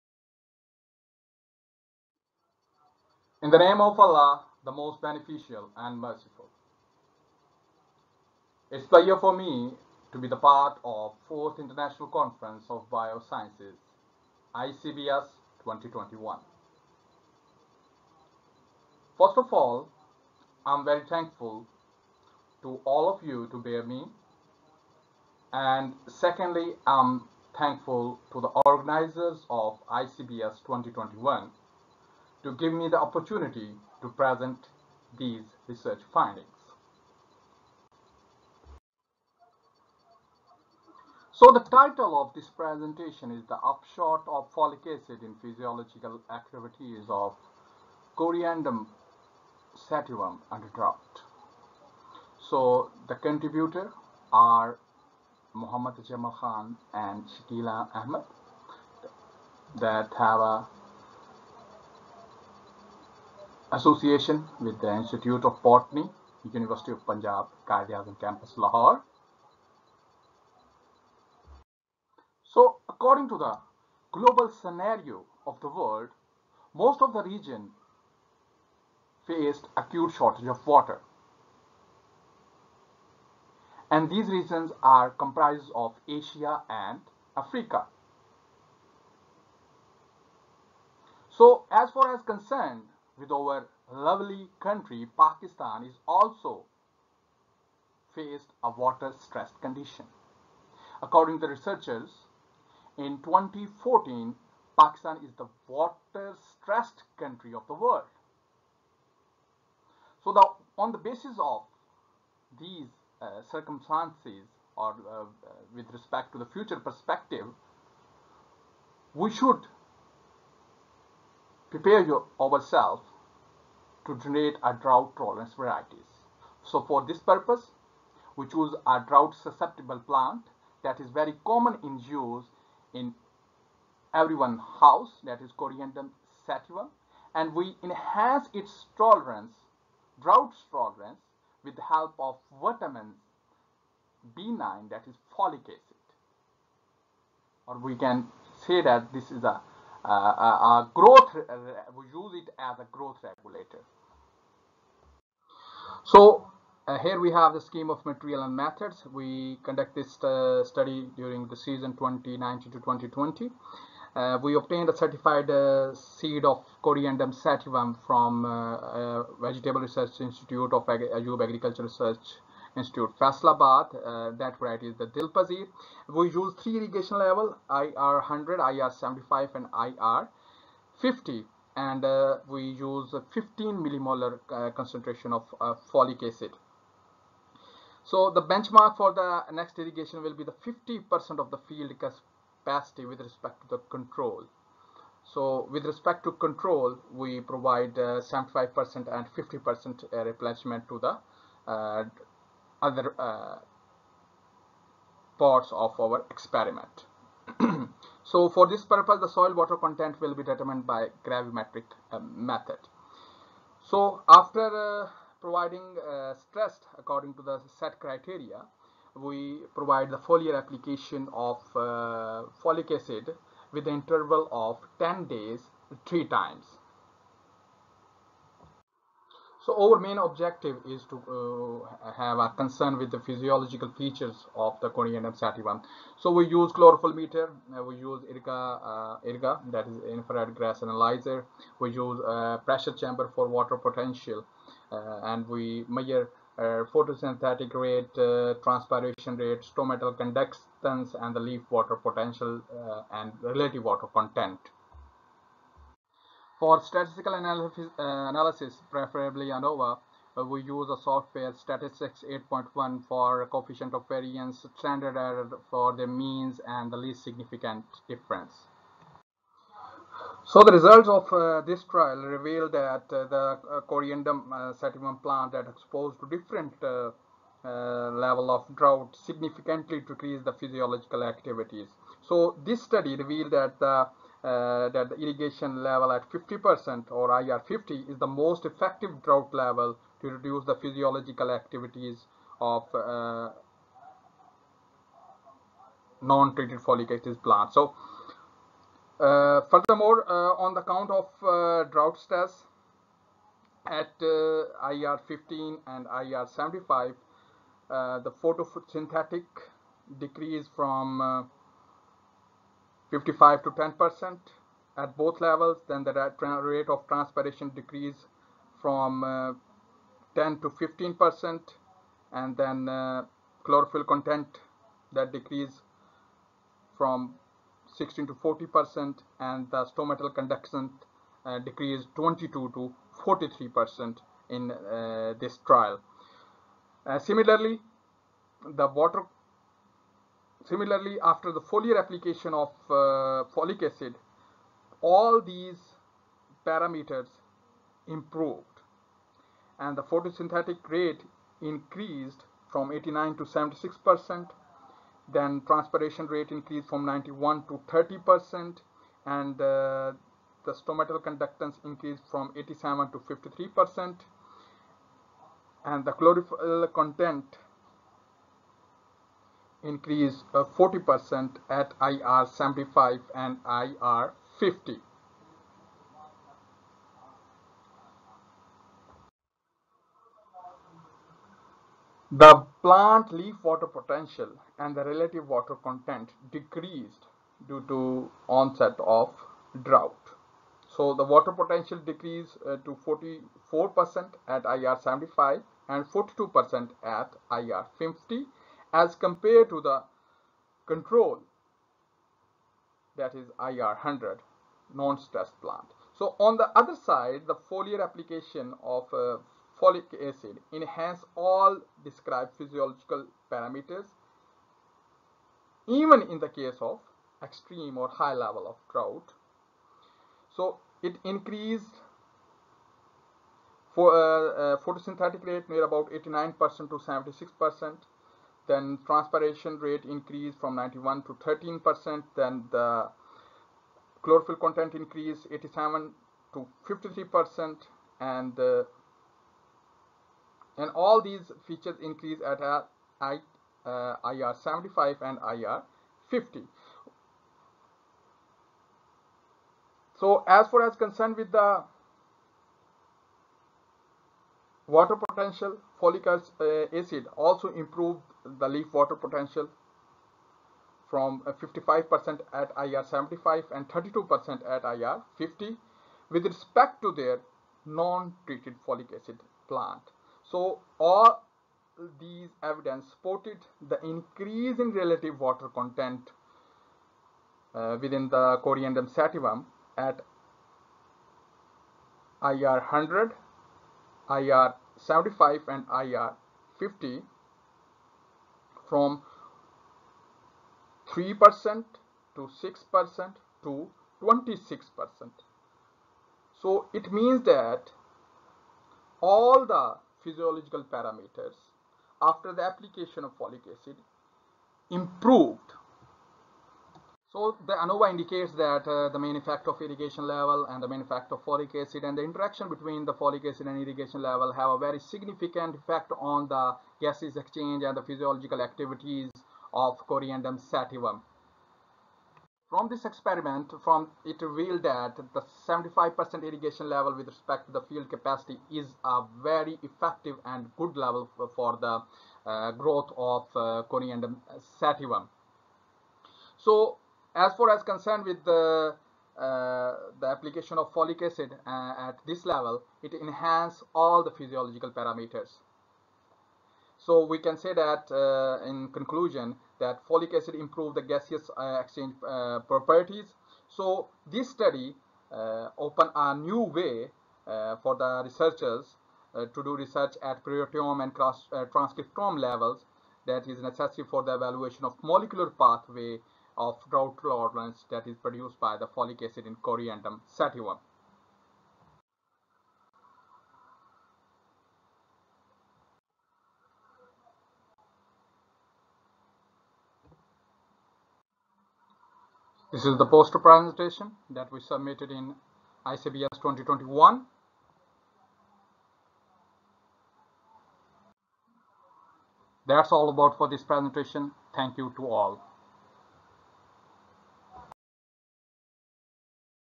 Physiological activities of Coriandrum sativum under drought | Miracle Herbal Diversity
Scientist from university of Punjab will give their research presentation.